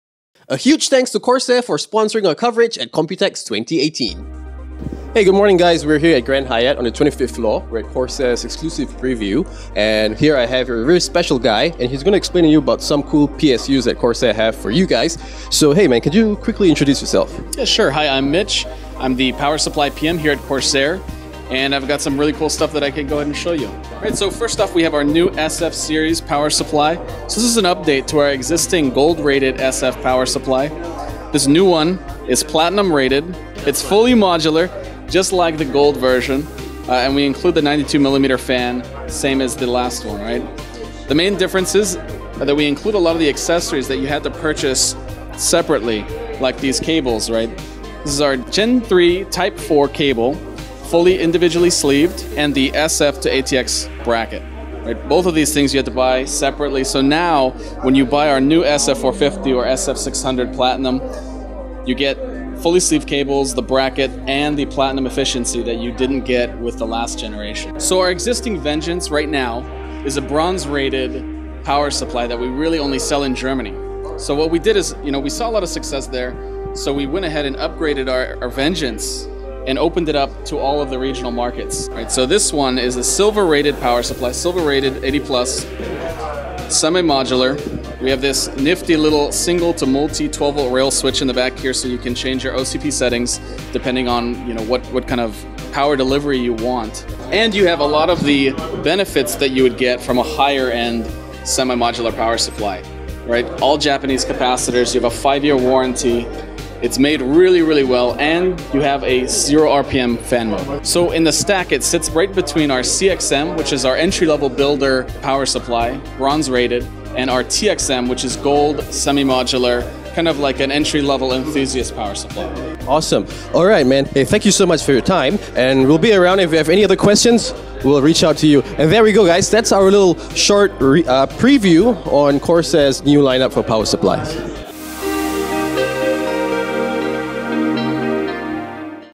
Corsair SF and Vengeance Series PSU - Computex 2018 Product Highlights
A quick look at Corsair’s new SF and Vengeance Series PSU.

Corsair’s new SF Series is fully modular like the previous version and it comes with a 92mm fan. This time, not only has Corsair included their Gen 3 Type 4 fully individually sleeved cables but they’ve also included the SF to ATX bracket. So for those who are looking to buy Corsair’s new SF450 and SF600 Platinum PSU, you’re in for a treat!

On the other hand, Corsair’s Vengeance Series 650M is a 650W 80 Plus Silver Certified PSU. This new semi modular PSU comes with a 5-year warranty and Zero RPM fan mode. This is going to be perfect to medium budget workstation and gaming rigs.